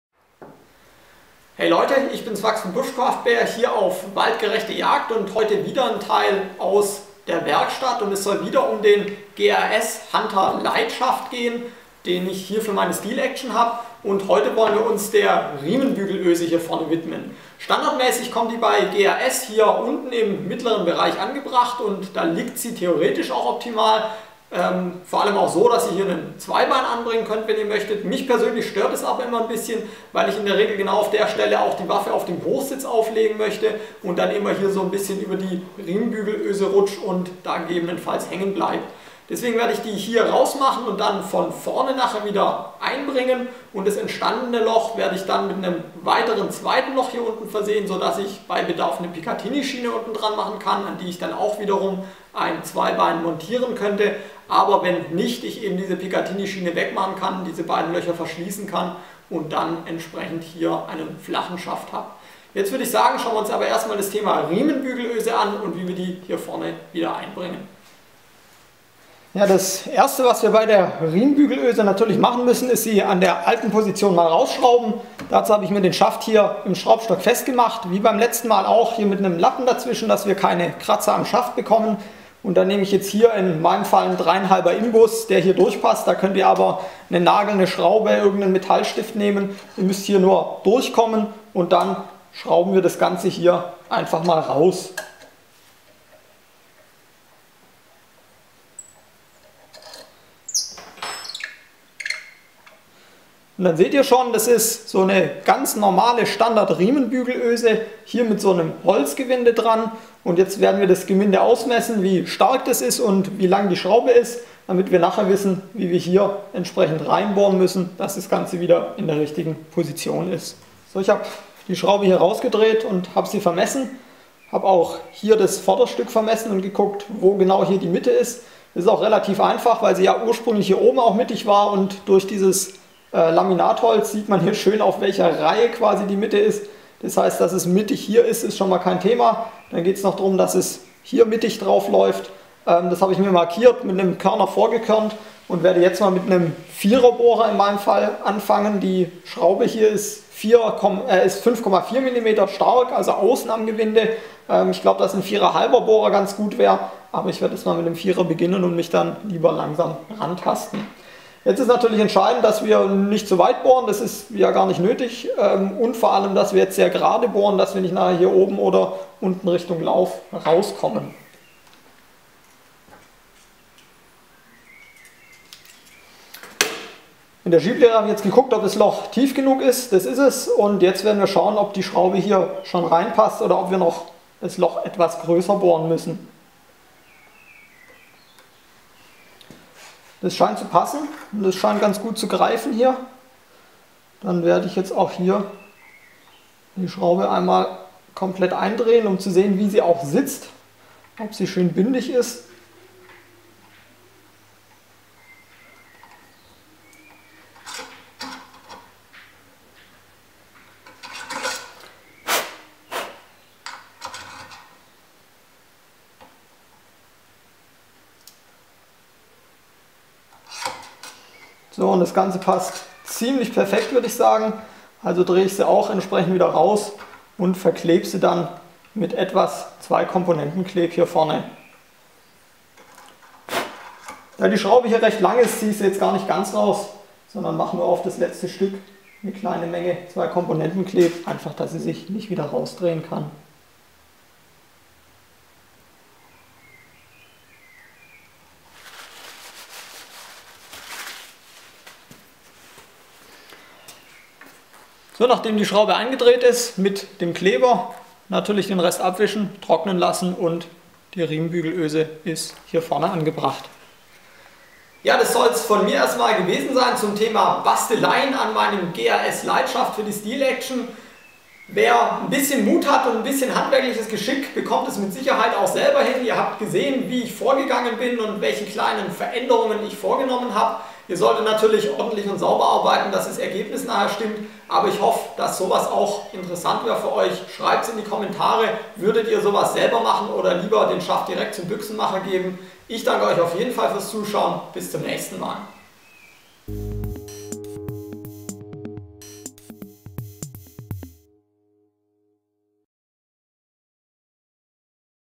GRS Hunter Light - DIY Riemenbügelöse versetzen
Heute zeige ich euch wie ich an meinem GRS Hunter Light die Riemenbügelöse an die Schaftspitze versetze.

-WERBUNG-